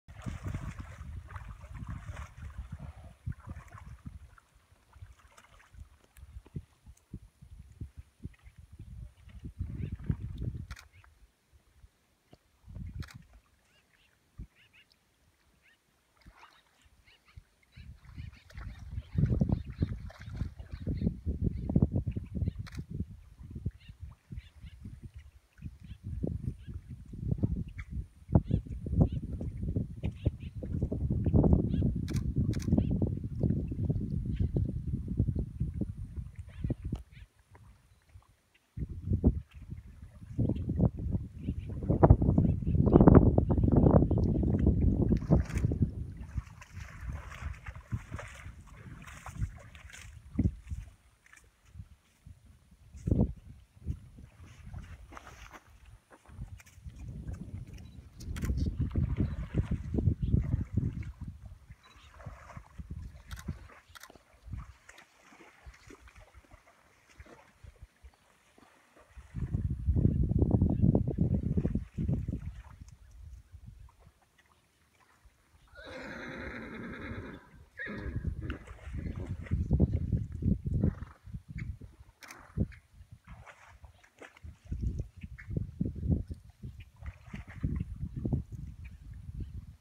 Saguaro Lake Wild Horses
Enjoying the lake on a hot August Morning